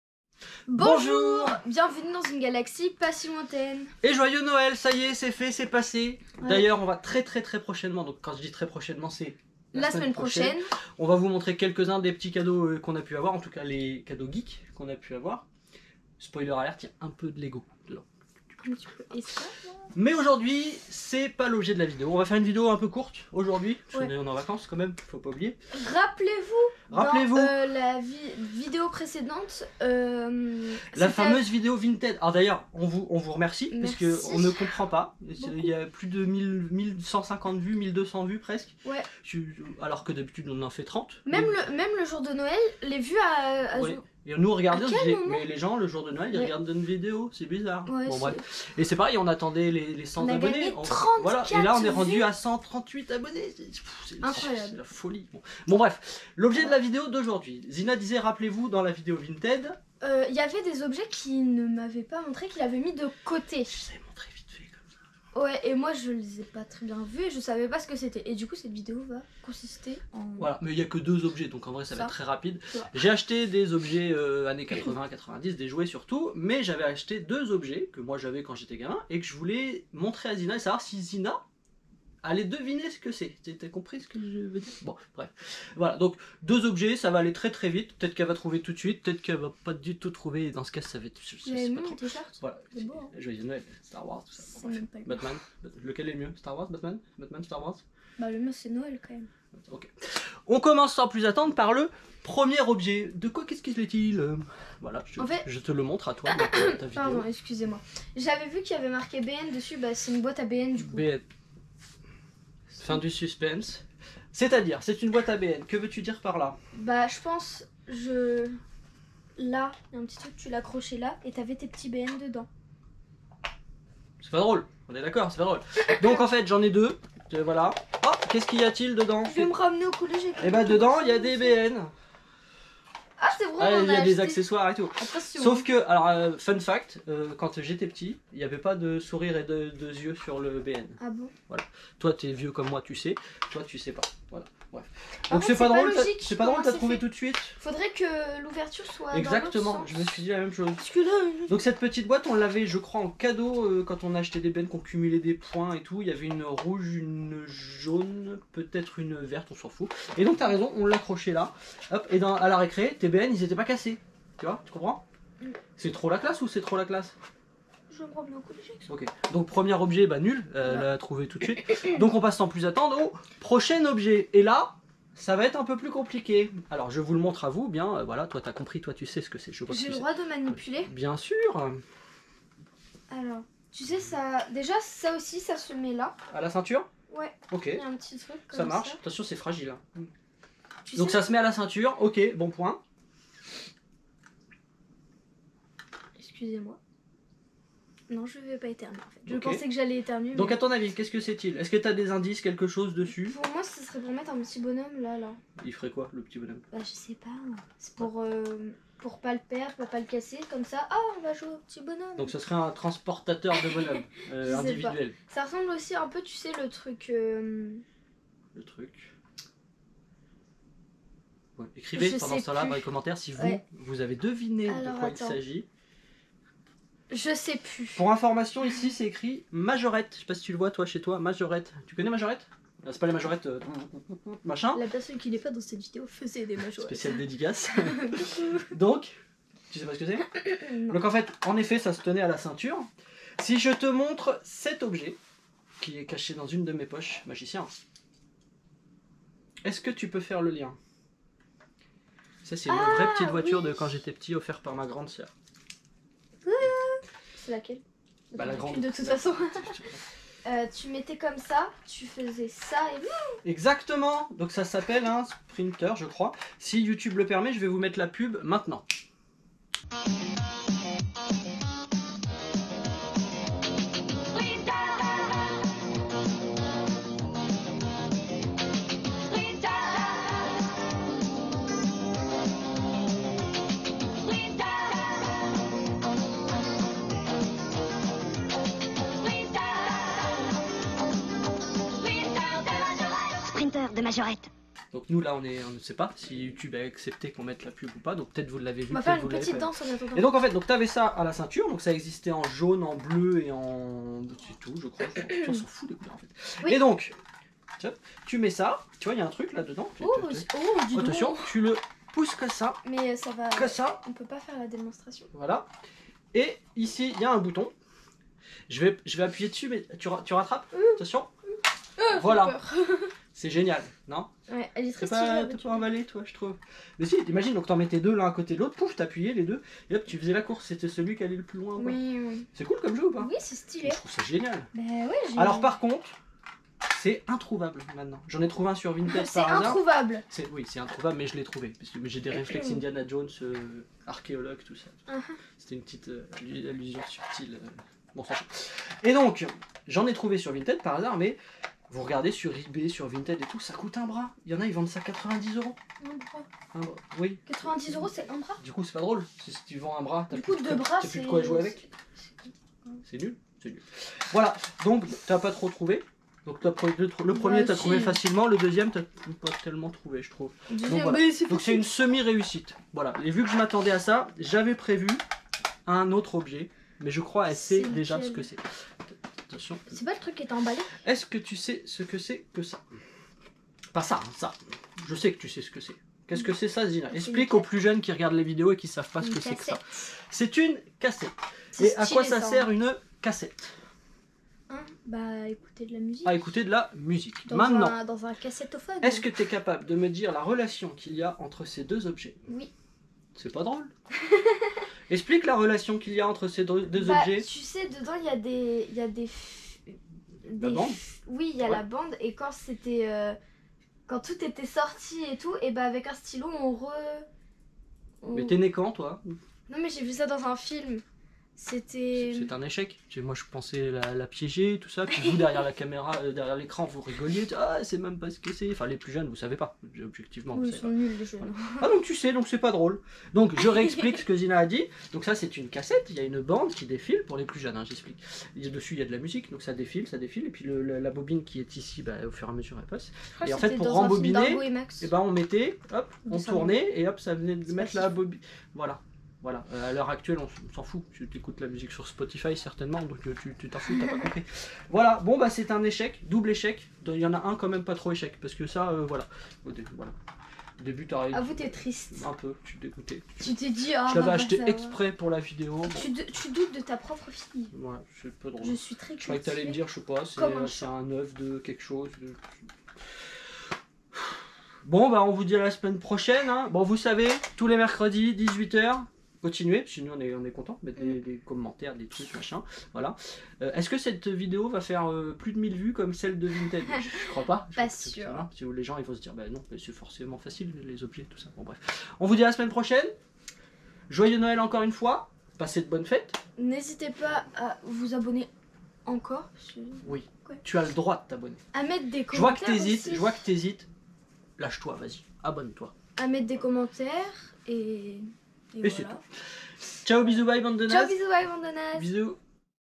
Elle devine des objets des années 80 !
Quelle est la relation qui lie un crayon et une K7 ? Et d'ailleurs, c'est quoi une K7 ?
Quelle est cette boite rouge que l'on accrochait à la ceinture ? Et cet œuf jaune qui était tellement classe dans la cours de récré ???